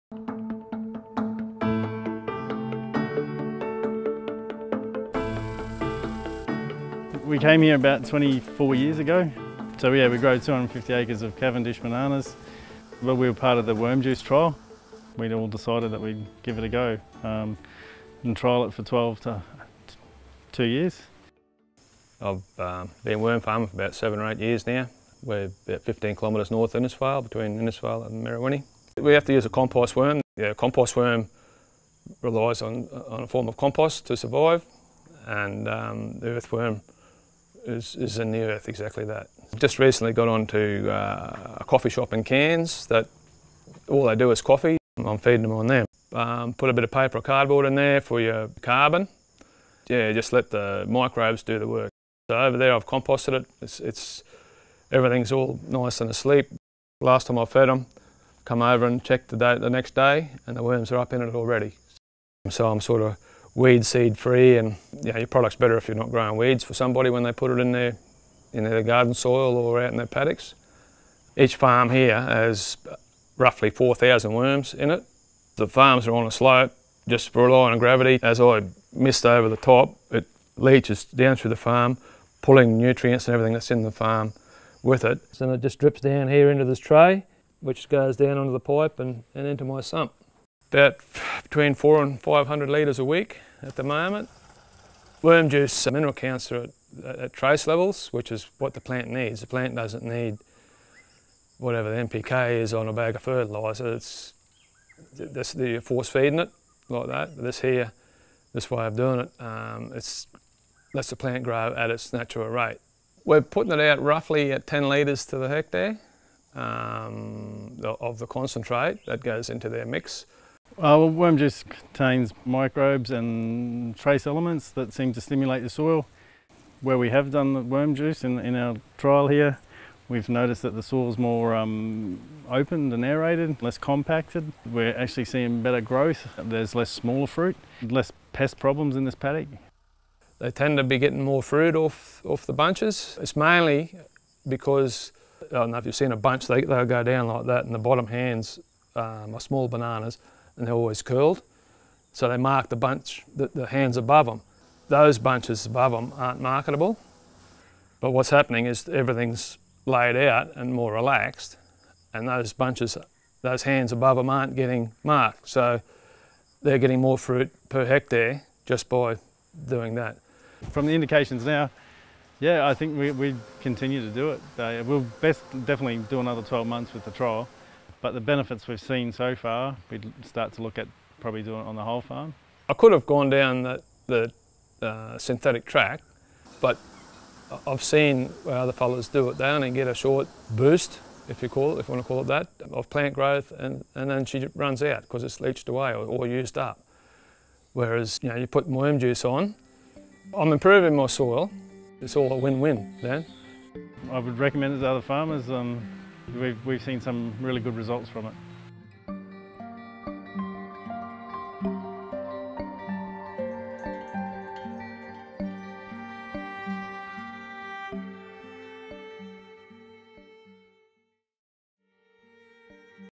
Mick Kruckow: Worm Juice Fertiliser for Farming
Vermiculturalist Mick Kruckow has an industrial size worm farm in Far North Queensland. He supplied worm juice to other farmers nearby as a trial. One of the farmers, banana grower Dereck Devaney, says it made a big difference to his soil health and crop growth.